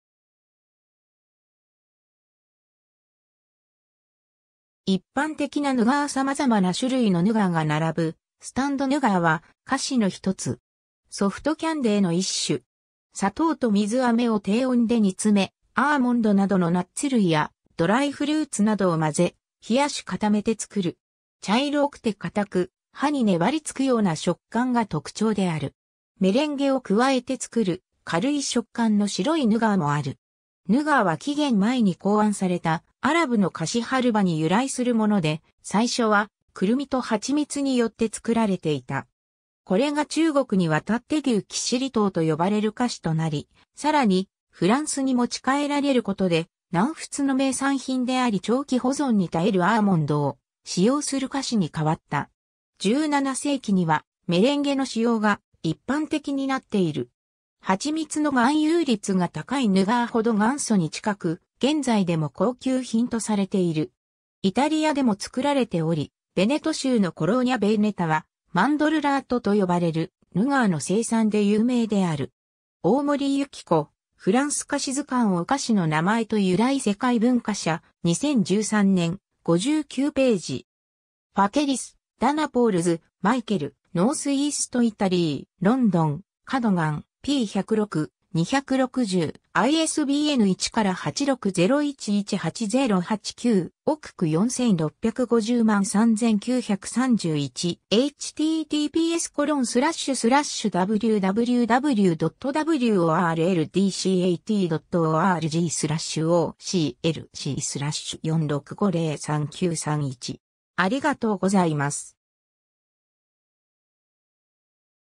ヌガー
キャンディ_(菓子)
一般的なヌガー 様々な種類のヌガーが並ぶスタンド ヌガー（仏: nougat）は、菓子のひとつ。
ソフトキャンデーの一種。
 砂糖と水飴を低温で煮詰め、アーモンドなどのナッツ類やドライフルーツなどを混ぜ、冷し固めて作る。
茶色くて固く、歯に粘りつくような食感が特徴である。
 メレンゲ（泡立てた卵白）を加えて作る、軽い食感の白いヌガー（Nougat de Montélimar、ヌガー・ド・モンテリマール）もある。
 ヌガーは紀元前に考案されたアラブの菓子ハルヴァに由来するもので、最初はクルミと蜂蜜によって作られていた。
これが中国にわたって牛軋糖(ニュウガータン)と呼ばれる菓子となり、さらにフランスに持ち帰られることで、南仏の名産品であり長期保存に耐えるアーモンドを使用する菓子に変わった。
17世紀にはメレンゲの使用が一般的になっている。
蜂蜜の含有率が高いヌガーほど元祖に近く、現在でも高級品とされている。
 イタリアでも作られており、ヴェネト州のコローニャ・ヴェーネタはマンドルラートと呼ばれるヌガーの生産で有名である。
 ^ 大森由紀子 『フランス菓子図鑑 お菓子の名前と由来』 世界文化社、2013年、59頁。
^ Facaros, Dana; Pauls, Michael (2001). Northeast Italy. (2nd ed ed.). London: Cadogan. p. 106, 260. ISBN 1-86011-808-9. OCLC 46503931.